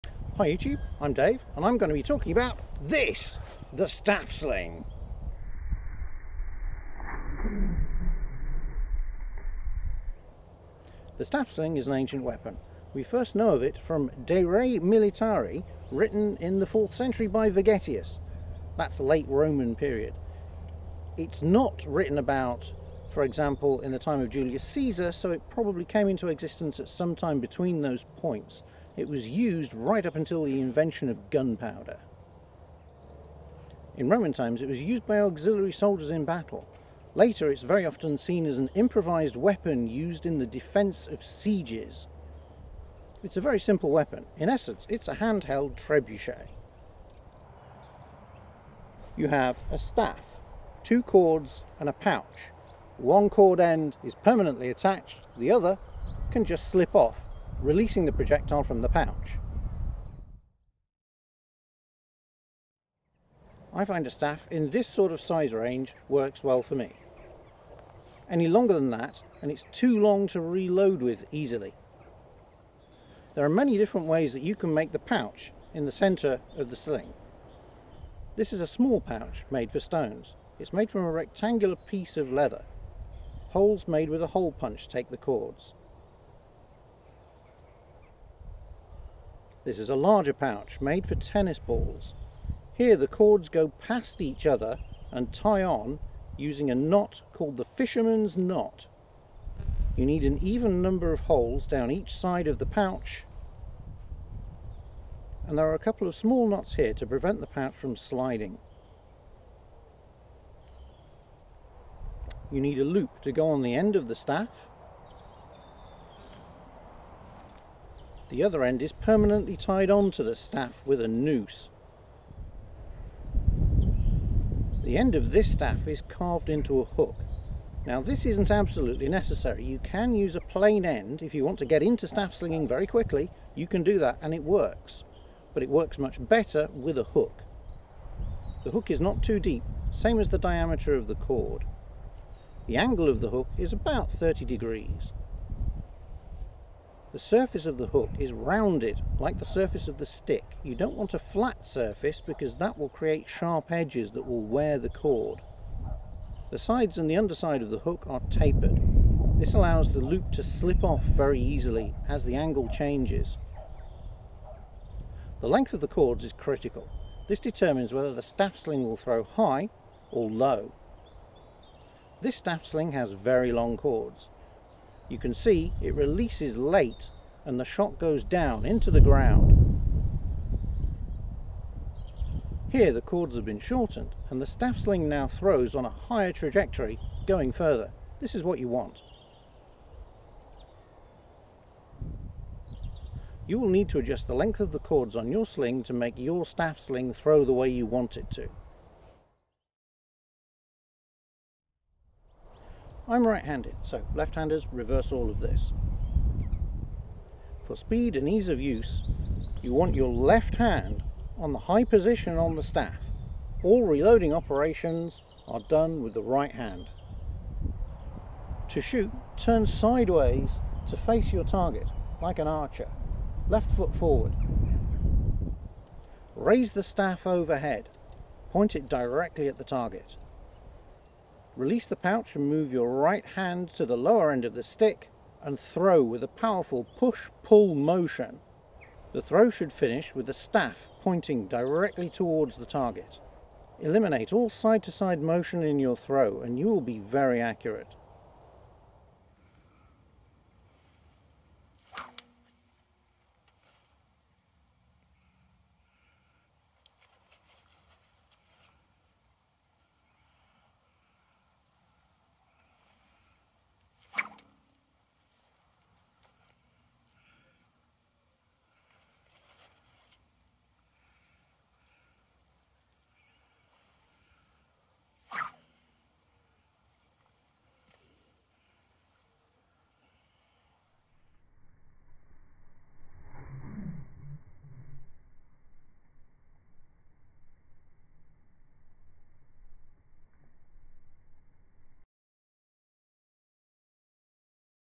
How to make and use the staff sling (fustibalus)
Staff slings are easy to make and use. The only limit to the size of rocks you can throw is your own strength. Once you try this, you will wonder how it was ever forgotten.